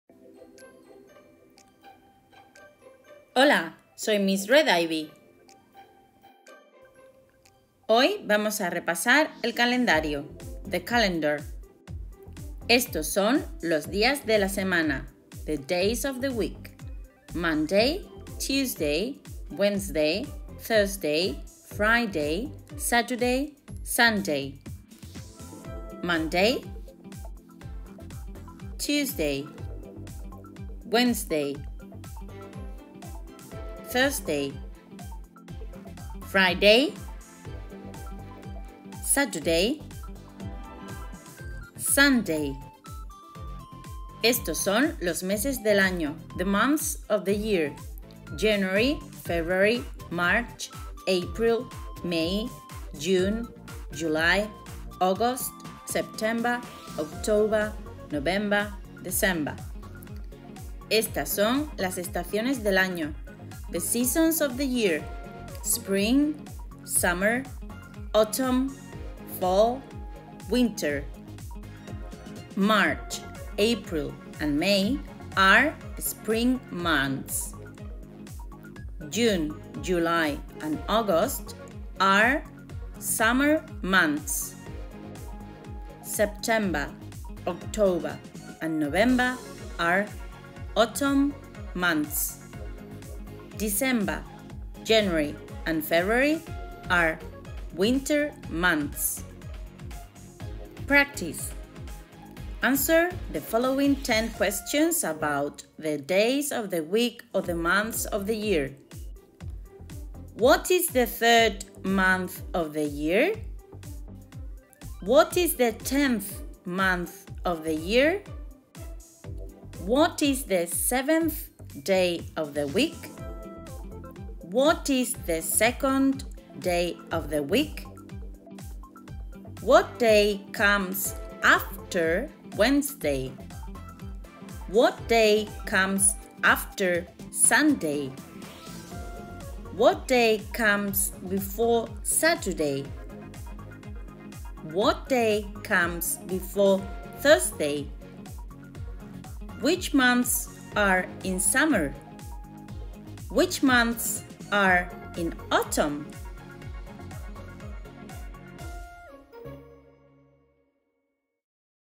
The Calendar
How to write the date in English / Cómo escribir la fecha en inglés